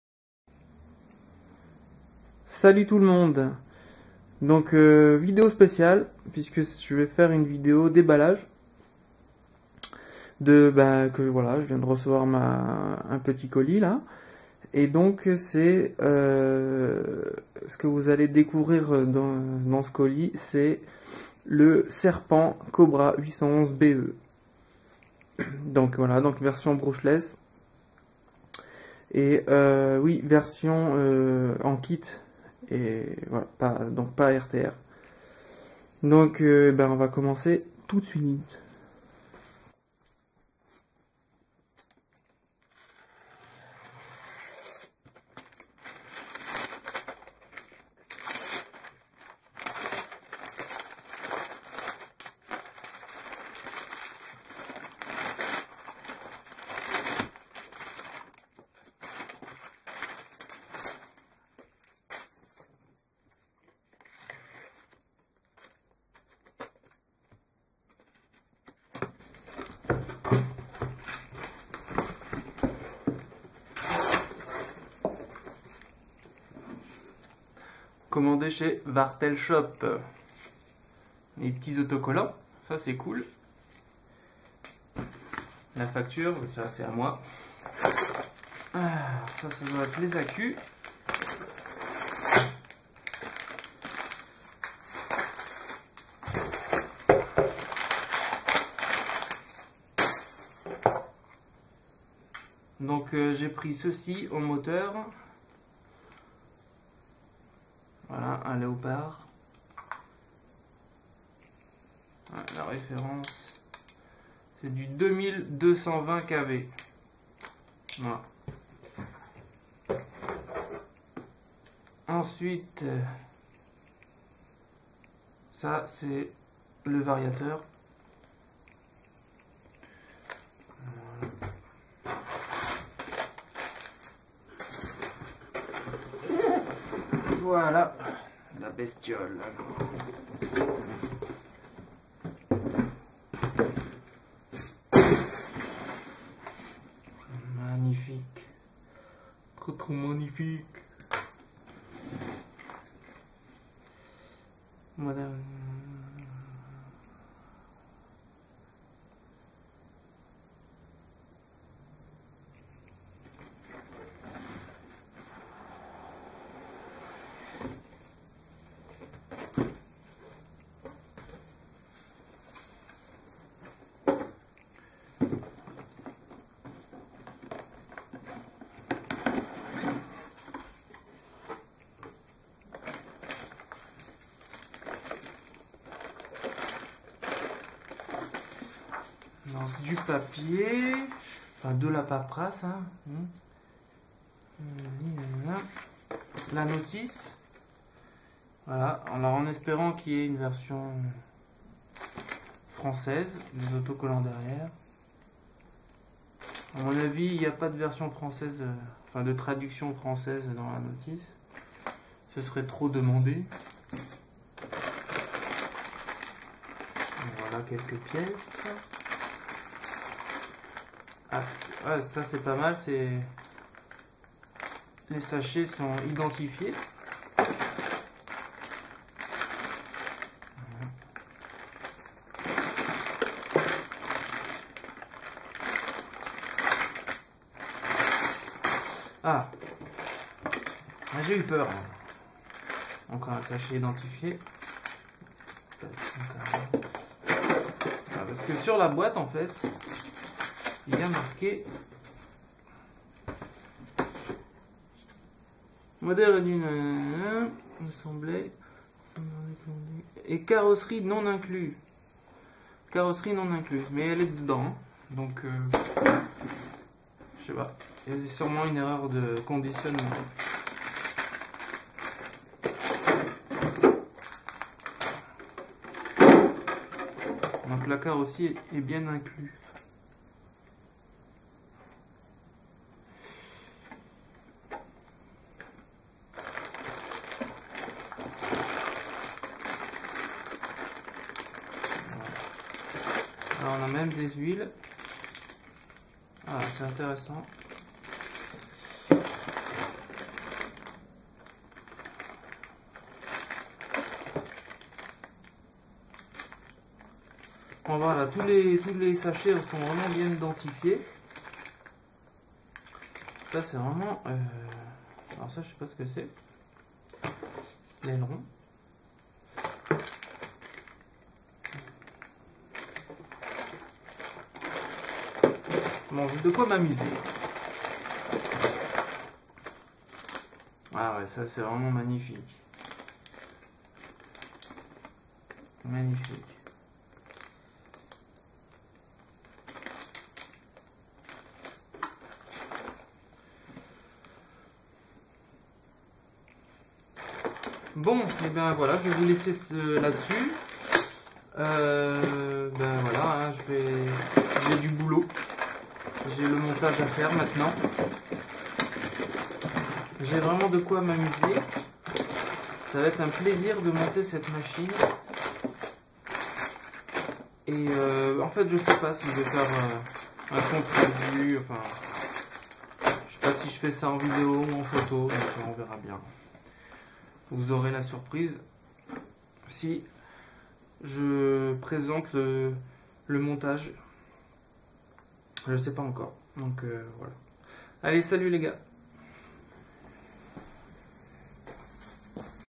Déballage du Serpent Cobra 811 BE
Déballage du buggy brushless de chez Serpent, le Cobra 811 BE.
La voiture est bien sûr livrée en kit.